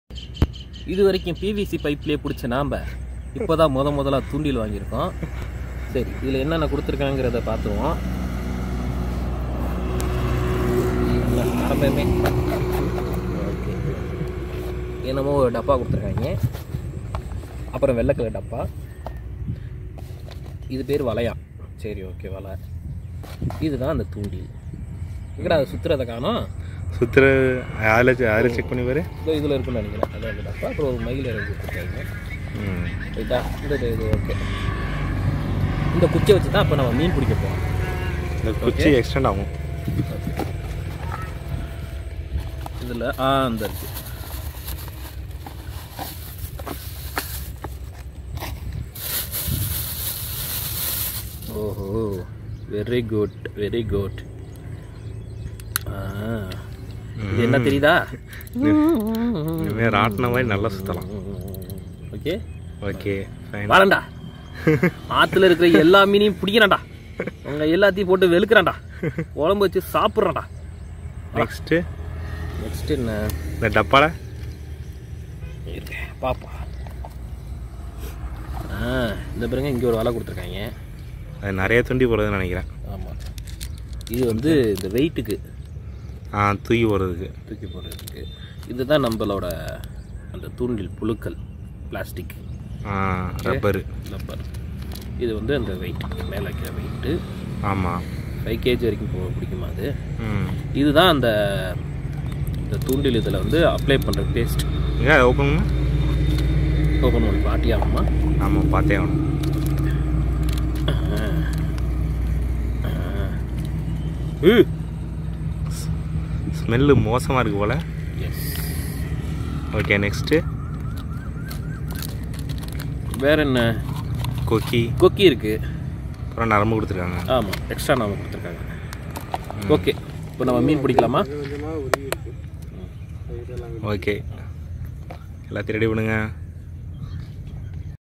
Best fishing rod unboxing tamil | Fishing rod setup for beginners | fishing rod review tamil status
Ordered fish rod rs 999 /-
given Best review.